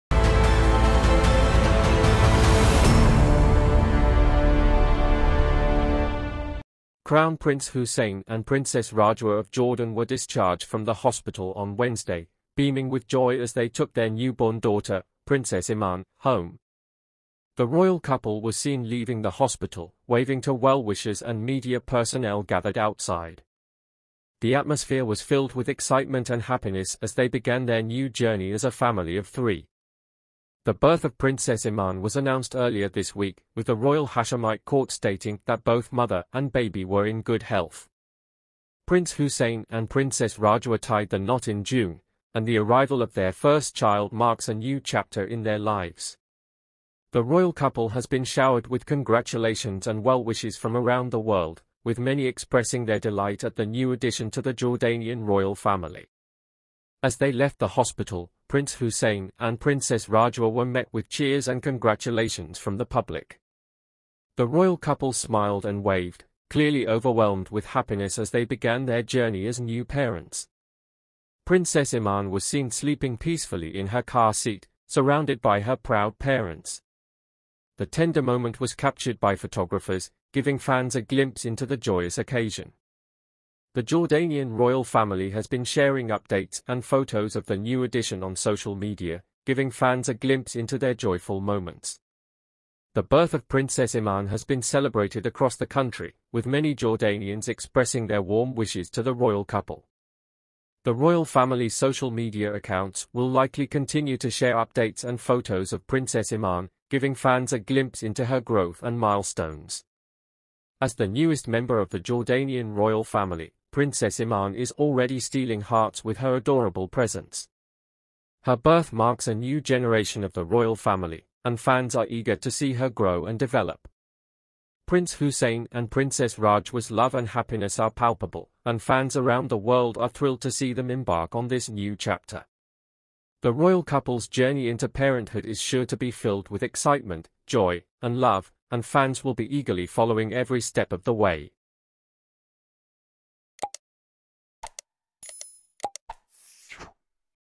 Prince hussein and rajwa Leave hospital Today with Cute Baby Iman
Jordan's Crown Prince Hussein and Princess Rajwa proudly take their newborn daughter home! Get the latest update on the royal family.